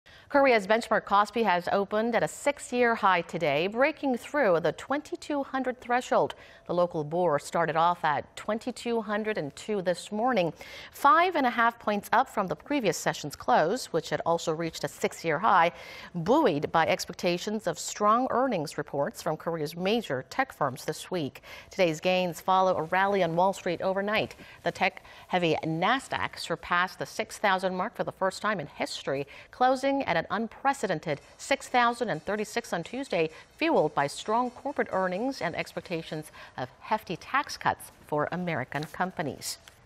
KOSPI opens at six-year high, breaking 2,200 threshold
KOSPI 2200선 돌파, 나스닥지수 사상 처음으로 6,000 돌파 마감
Korea's benchmark KOSPI has opened at a six-year-high today... breaking through the 22-hundred threshold.
The local bourse started off at 22-hundred-and-two this morning,... 5-and-a-half points up from the previous session's close, which had also reached a six-year-high,... buoyed by expectations of strong earnings reports from Korea's major tech firms this week.
Today's gains follow a rally on Wall Street overnight.
The tech heavy NASDAQ surpassed the 6-thousand mark for the first time in history,... closing at an unprecedented six-thousand-and-thirty-six on Tuesday, fueled by strong corporate earnings and expectations of hefty tax cuts for American companies.